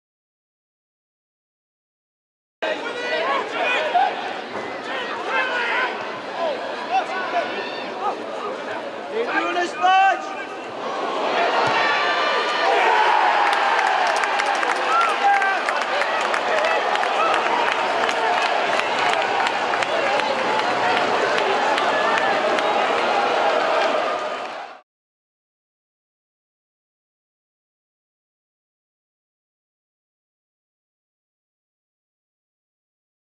Tom Nichols scores against Torquay | Exeter City Football Club
Exeter City striker Tom Nichols scores a belter against local rivals Torquay United at Plainmoor to open the scoring.  It ended 3-1 to the Grecians, as David Wheeler and Alan Gow scored either side of Karl Hawley's goal for the hosts.